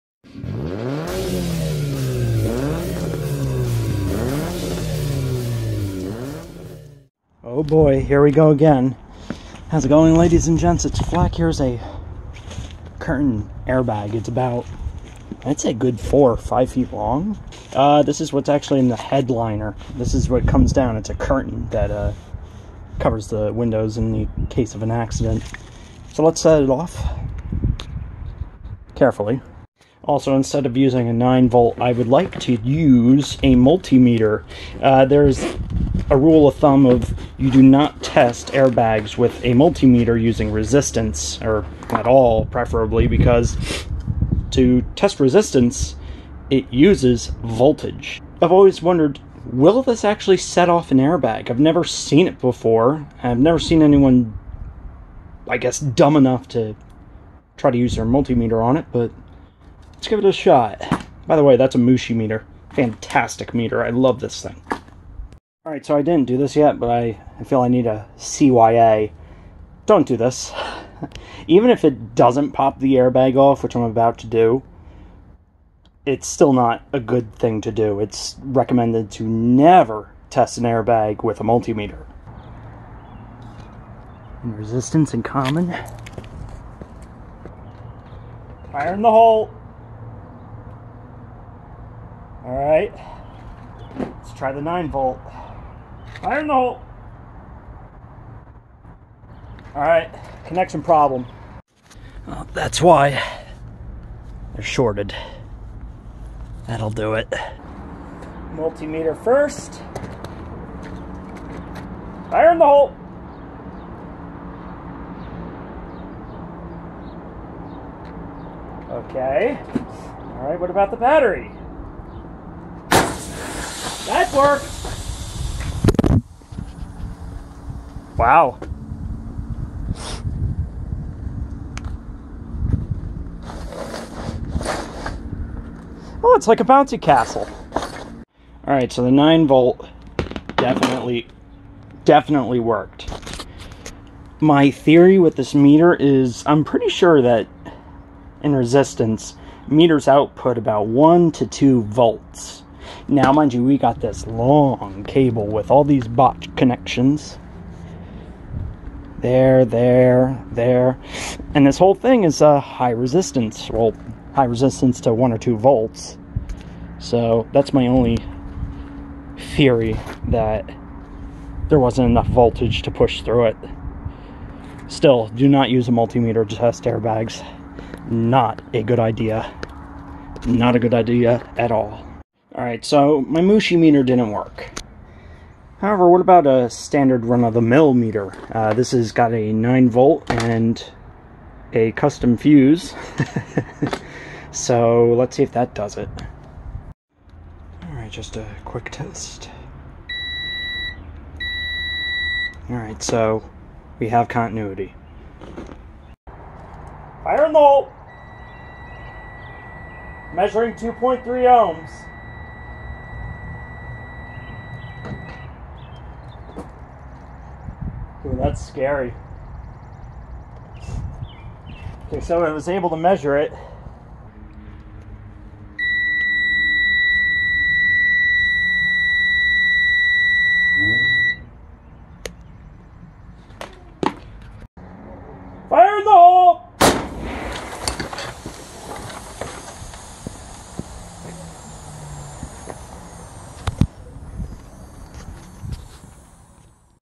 Multimeter testing an airbag: will it go off?
A rule of thumb: NEVER test an airbag with a multimeter. But what happens if you do?
I used 2 DVOM's, one of which being a Mooshimeter, and one being a more "run of the mill", standard multimeter.